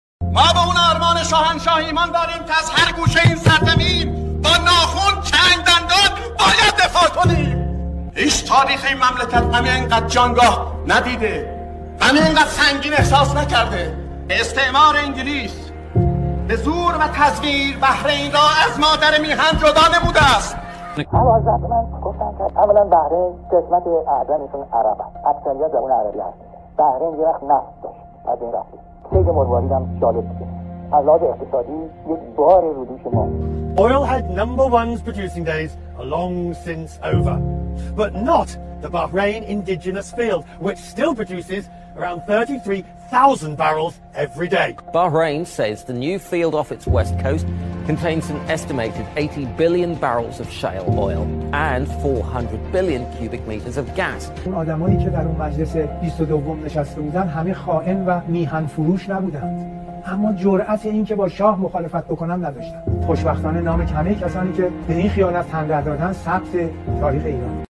🎥صحبت‌های تند نماینده‌ی مجلس زمان پهلوی در مخالفت با حماقت شاه و جدایی بحرین از میهن🔹️
🎥یادی کنیم از صحبت‌های تند نماینده‌ی مجلس زمان پهلوی در مخالفت با حماقت شاه و جدایی بحرین از میهن

🔹️ خیانت بزرگی که این روزا توسط کانالای سلطنت طلب به عنوان پیروزی تاریخی ازش یاد میشه